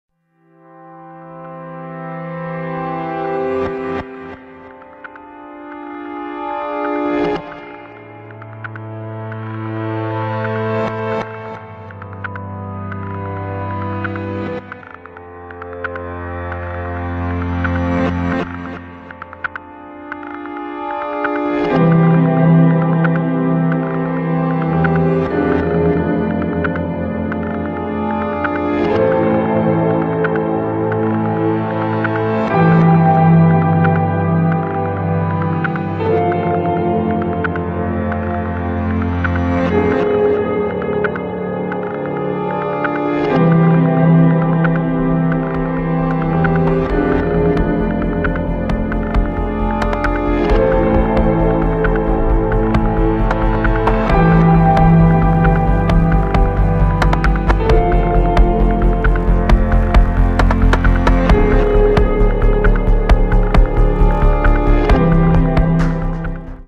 Real Estate Promo Highlight Photo Real for Website - Drone Images
This is an example of just a photo reel of nice WNY locations and homes that an agent can highlight on their website or other social media outlets.